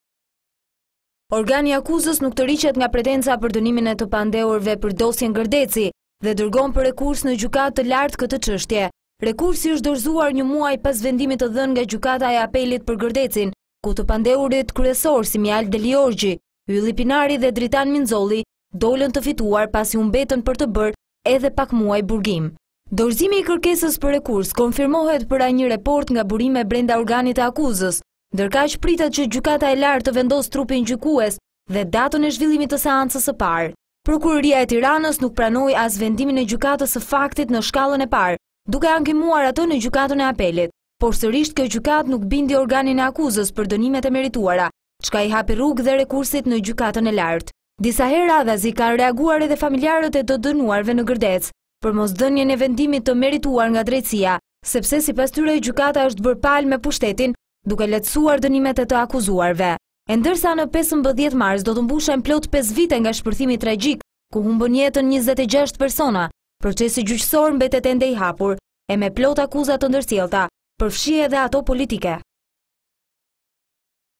Tragjedia e Gërdecit, Prokuroria çon dosjen në Gjykatën e Lartë
TIRANE- Prokuroria e Tiranës dërgon në Gjykatën e Lartë vendimin e Gjykatës së Apelit për Gërdecin.
Prokuroria i bëri rekurs vendimit të 13 shkurtit të 2013 të Gjykatës së Apelit e cila vendosi të reduktojë dënimin për tre prej të akuzuarve kryesorë të tragjedisë së Gërdecit: Ylli Pinari nga 18 vjet u dënua me 15 vjet burg, ndërsa Dritan Minxhollit dhe Mihal Delijorgjit iu reduktua dënimi me 1/3.
(ad.ti/Shqiptarja.com)